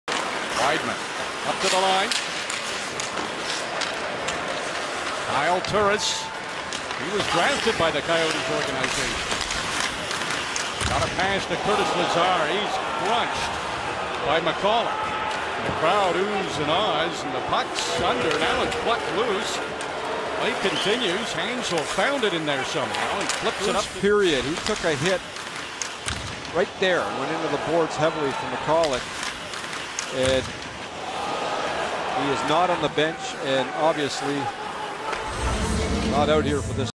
Curtis Lazar Injured After Shove Into the Boards (10/24/15)
Curtis Lazar Injured After Shove Into the Boards (10/24/15) Injury Michalek